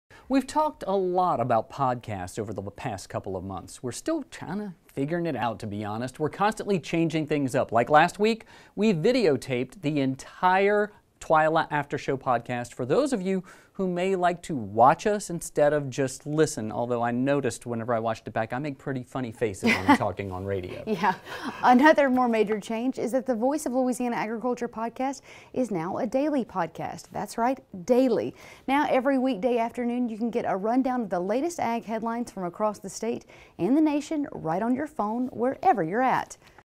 Voice of Louisiana Agriculture Daily Podcast
We have good news for all you podcast fans!  The Voice of Louisiana Agriculture podcast is now a daily podcast -- that's right -- daily.  Now, every weekday afternoon, you can get a rundown of the latest ag headlines from across the state and the nation right on your phone, wherever you're at!!

Want to see more videos about Louisiana farmers and the food they grow?